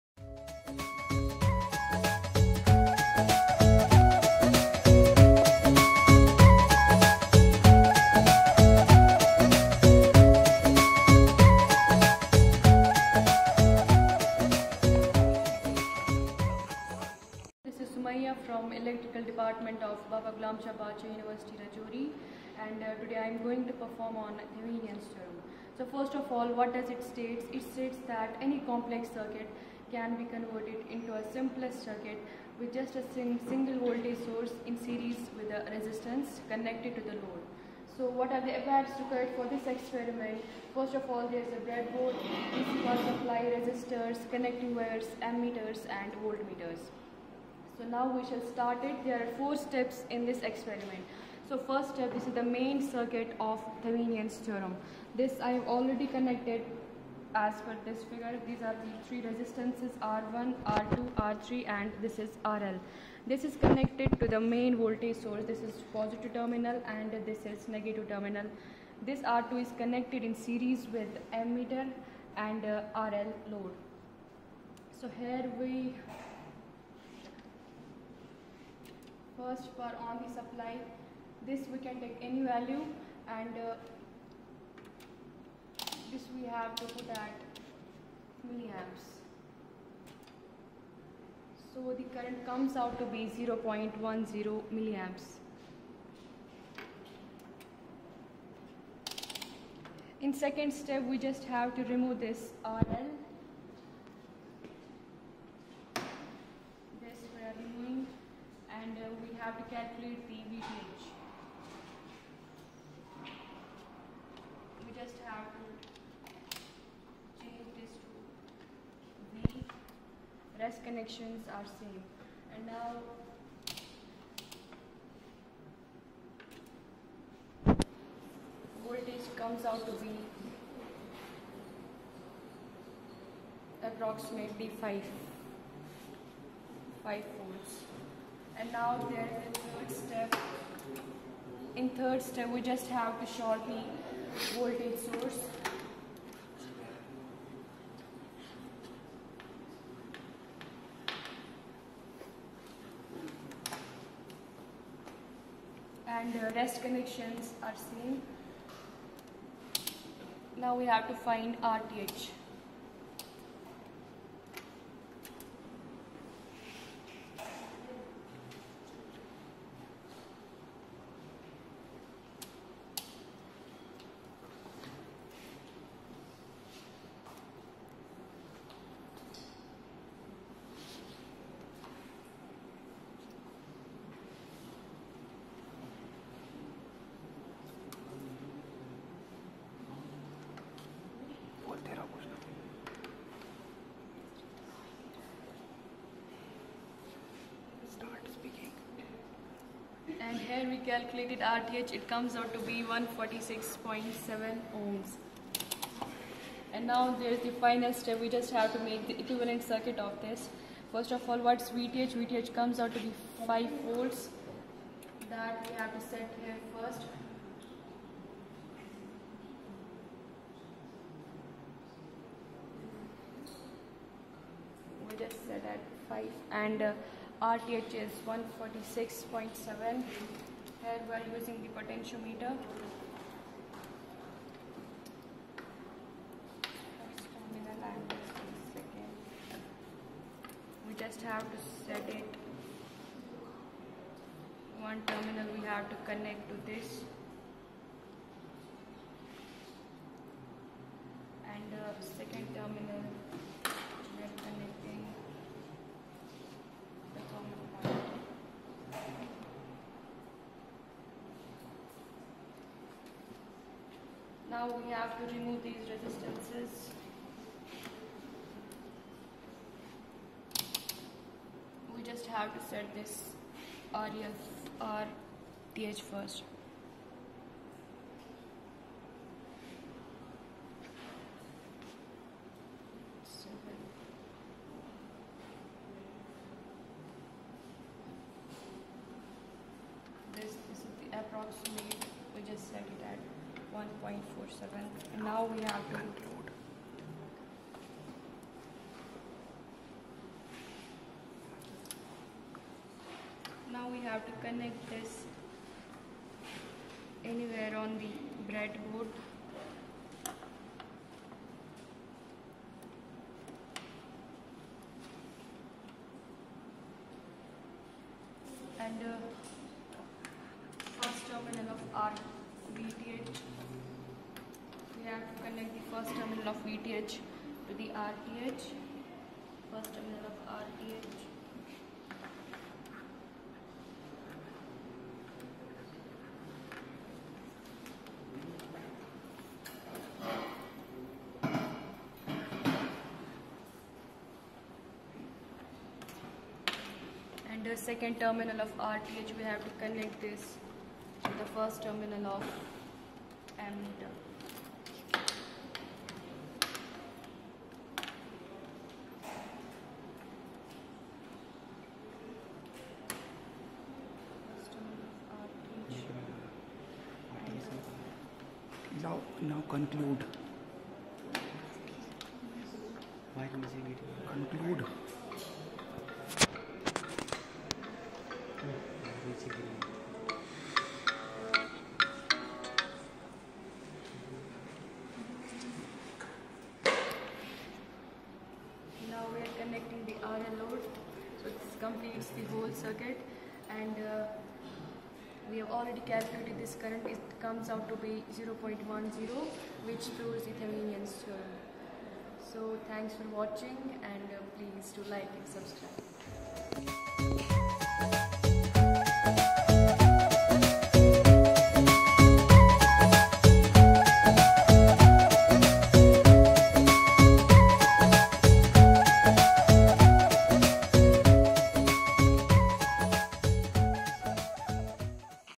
Thevenin's Theorem | EREE | SOET | BGSBU
• Thevenin’s Theorem states that “Any linear circuit containing several voltages and resistances can be replaced by just one single voltage in series with a single resistance connected across the load“.

• In other words, it is possible to simplify any electrical circuit, no matter how complex, to an equivalent two-terminal circuit with just a single constant voltage source in series with a resistance (or impedance) connected to a load.

• Thevenin’s Theorem is especially useful in the circuit analysis of power or battery systems and other interconnected resistive circuits where it will have an effect on the adjoining part of the circuit.

• As far as the load resistor RL is concerned, any complex “one-port” network consisting of multiple resistive circuit elements and energy sources can be replaced by one single equivalent resistance Rs and one single equivalent voltage Vs. Rs is the source resistance value looking back into the circuit and Vs is the open circuit voltage at the terminals.